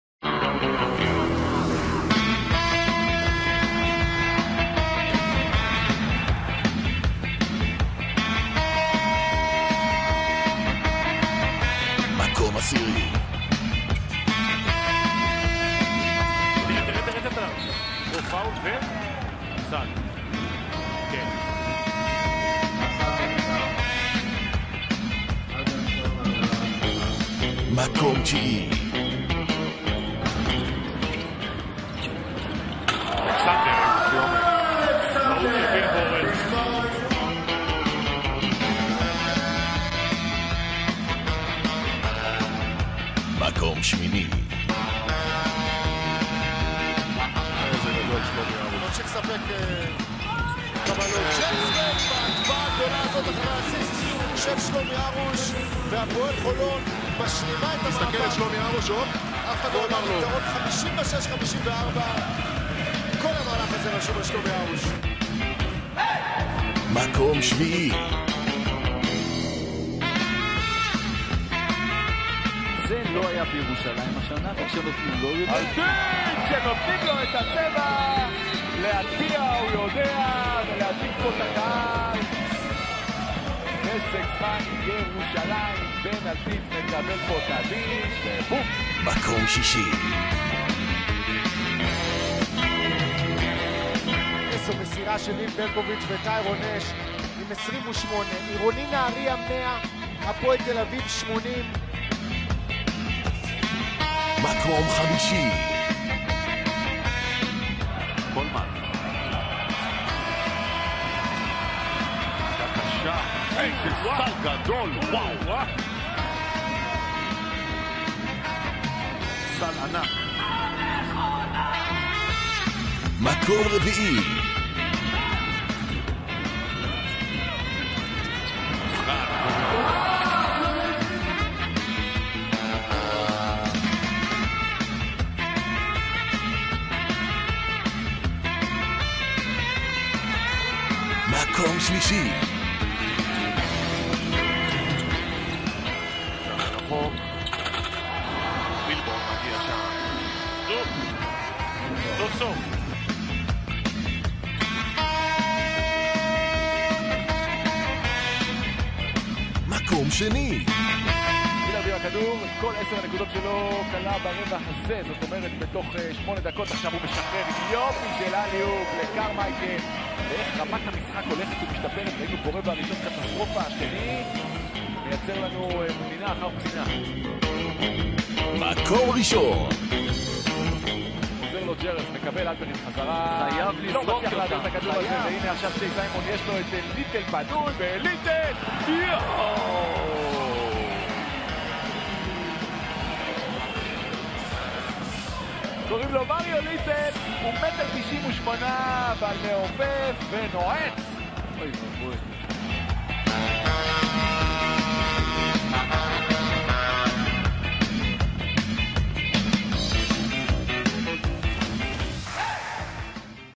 טופ 10 - מחזור 26
עשרת המהלכים היפים של מחזור 26 בליגת ווינר סל. עונת 16/17